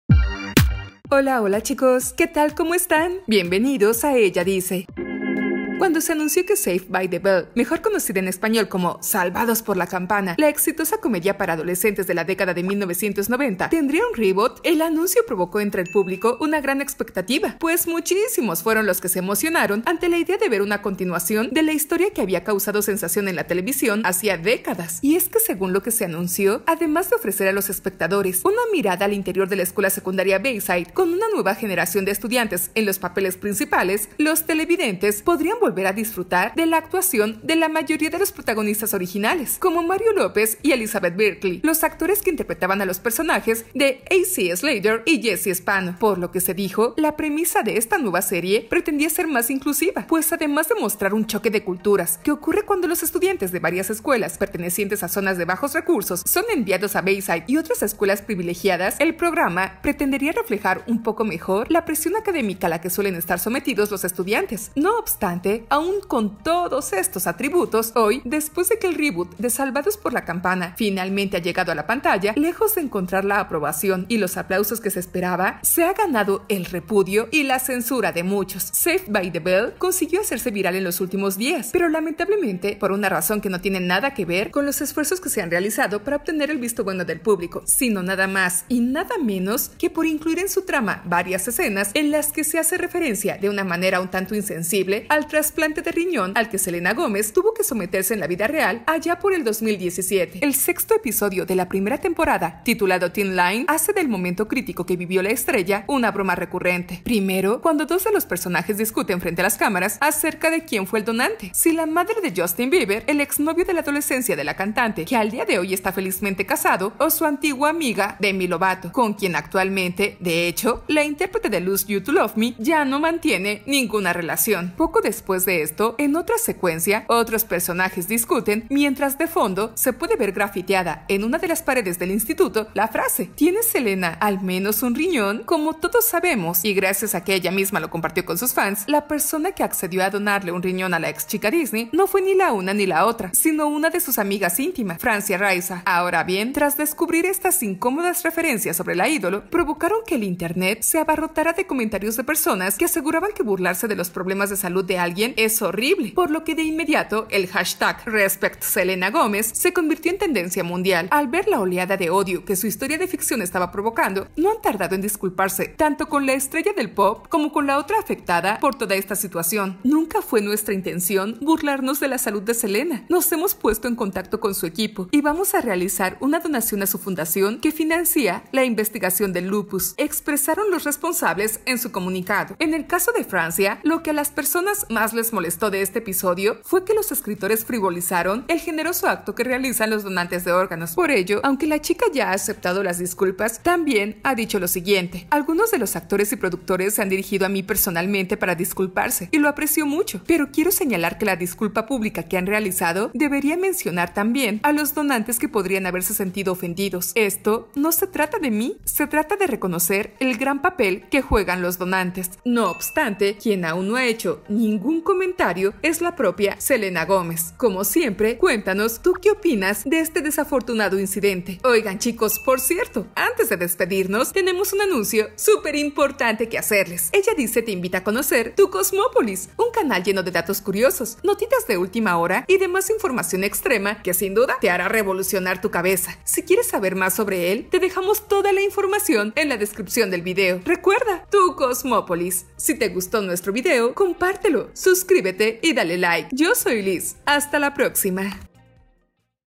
Nueva Serie De TV Es ARRASADA Por BURLARSE Del TRASPLANTE De RIÑÓN De SELENA GOMEZ...Por Publicidad?
Nuevo SALVADOS X LA CAMPANA Desata IRA En La RED Al BURLARSE Del TRASPLANTE De RIÑÓN De SELENA GOMEZ

Ya somos más de  1.17 M almas suscritas y unidas en Ella Dice 1.17 M